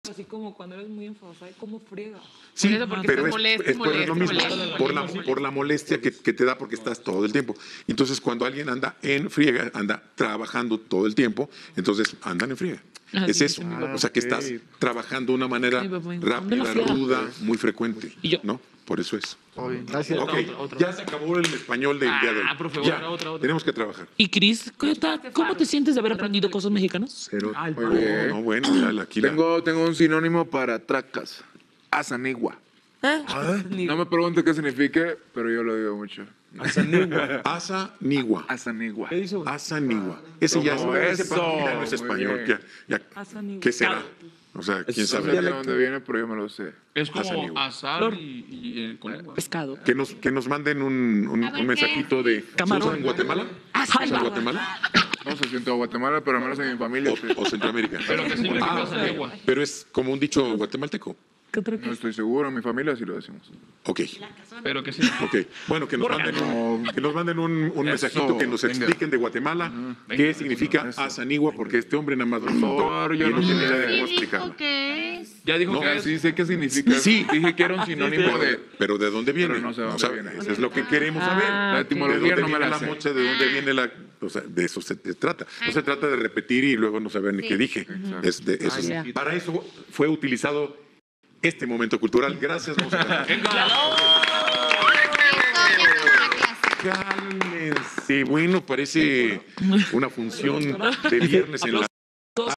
Cristian Salguero dice ASA NIGUA, pero no sabe el significado
“Asa Nigua”, que es una expresión de elogio